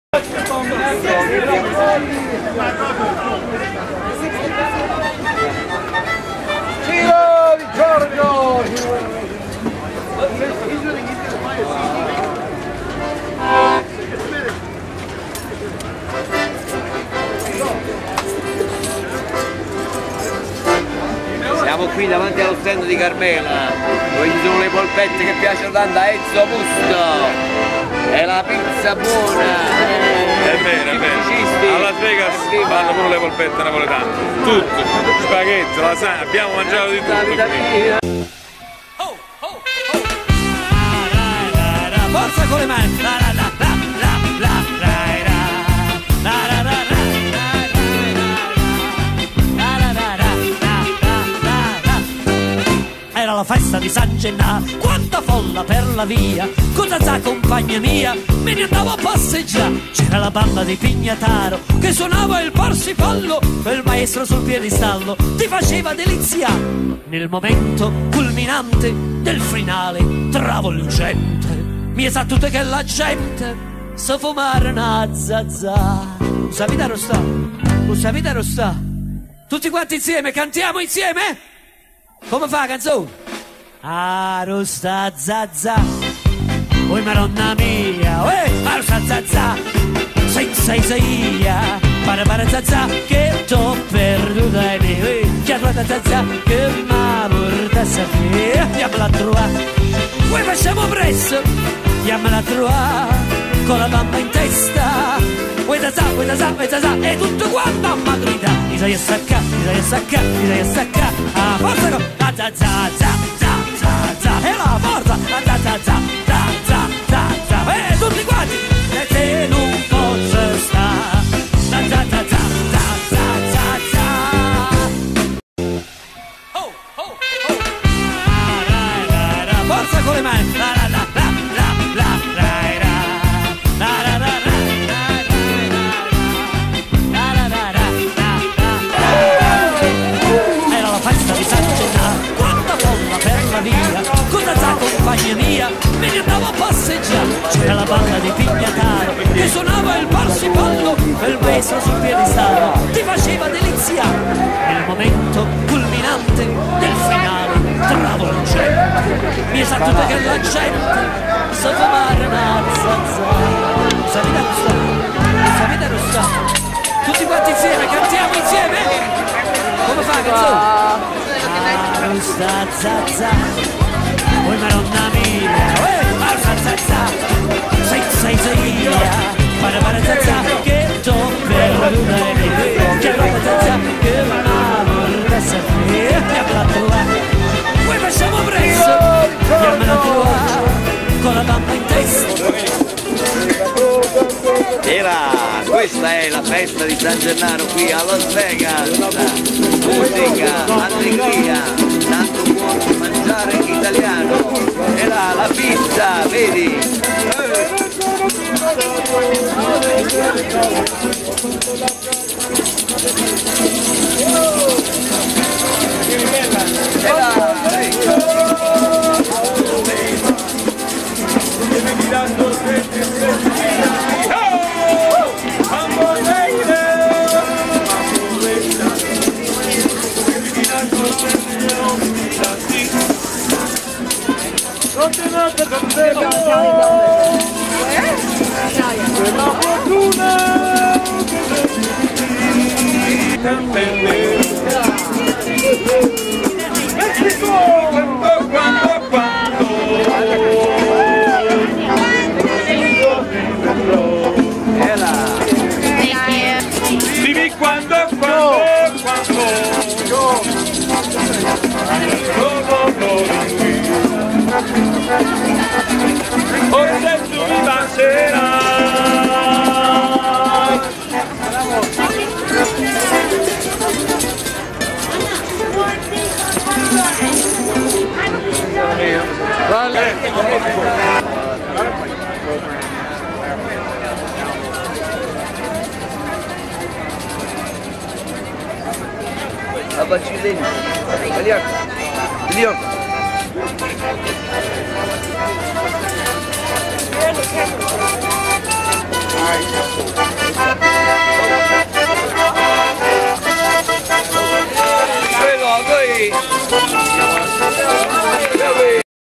Pulcinella a Las Vegas San Gennaro Feast 16 sett 2007
Pulcinella a Las Vegas San Gennaro Feast 16 sett 2007 prima parte con Ciro Giorgio, Enzo Musto,Angelo De Rosa,Emilio Baglioni e Carlos Velasco